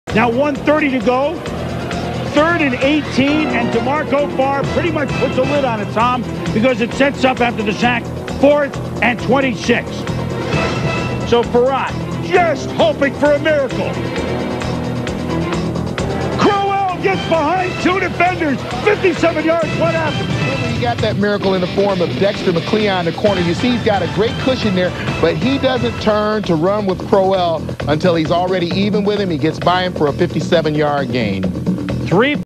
Lions convert on 4th and 26 - Rams @ Lions 1999
Lions would go on to win the game